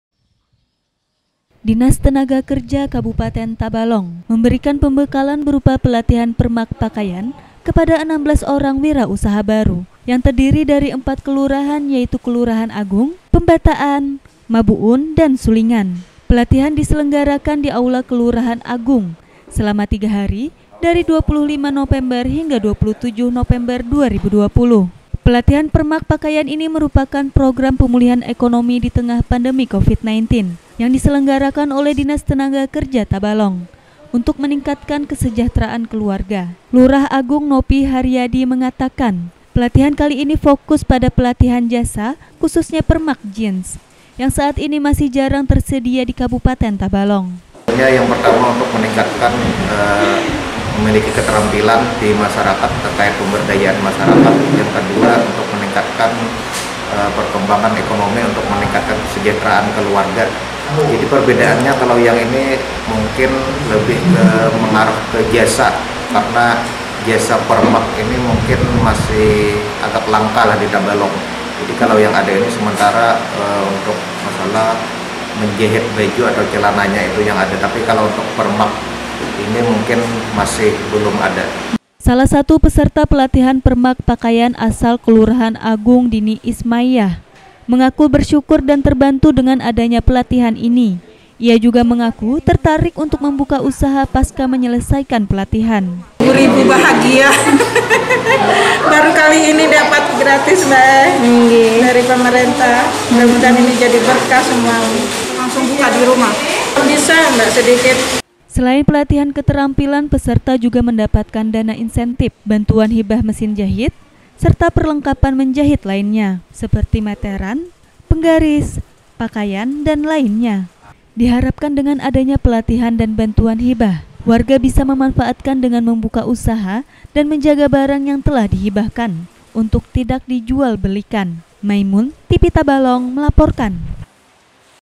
Disnaker Berikan Pelatihan Permak Pakaian, Khususnya Jeans Yang Jarang Tersedia di Tabalong
Sebanyak 16 Orang Wira Usaha Baru Yang Terdiri Dari 4 Kelurahan, Yakni Kelurahan Agung, Pembataan, Mabuun, Dan Sulingan, Mengikuti Pelatihan Berupa Permak Pakaian. Pelatihan Tersebut Merupakan Bagian Dari Program Pemulihan Ekonomi Ditengah Pandemi Covid-19 Yang Diselenggarakan Dinas Tenaga Kerja Tabalong, Untuk Meningkatkan Kesejahteraan Keluarga Pada 27 November 2020.